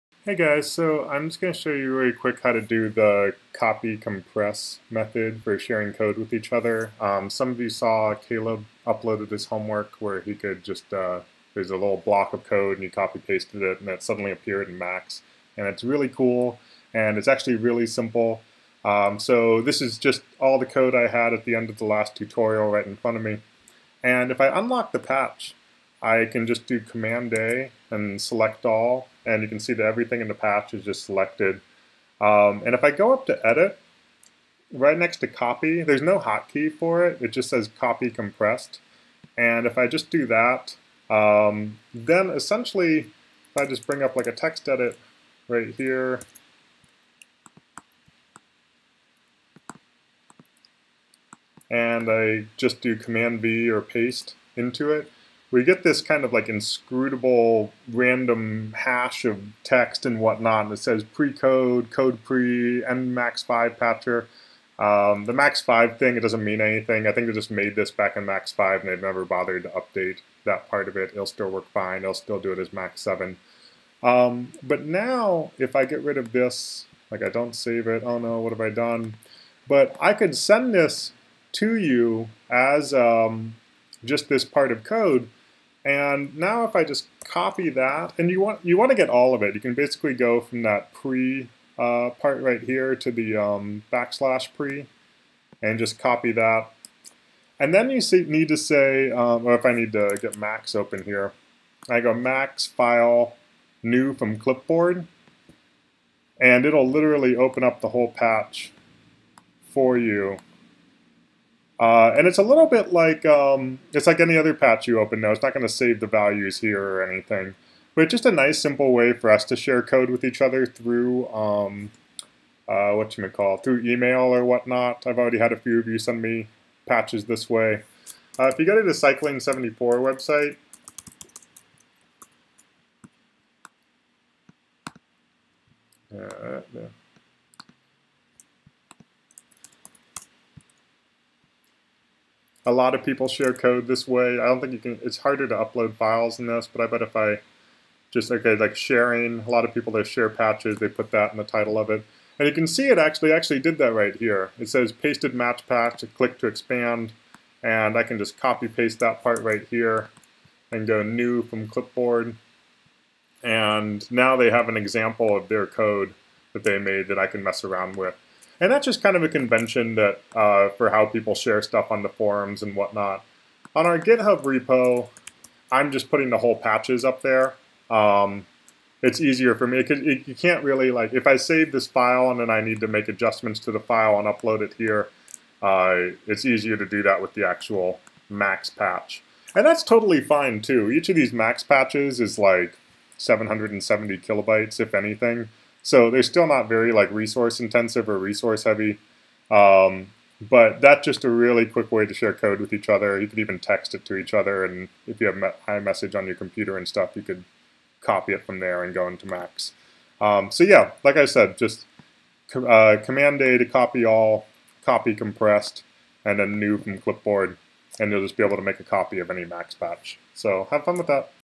Copy Compress for Sharing Max Patches
How to use the copy compress function to quickly share max patches with people.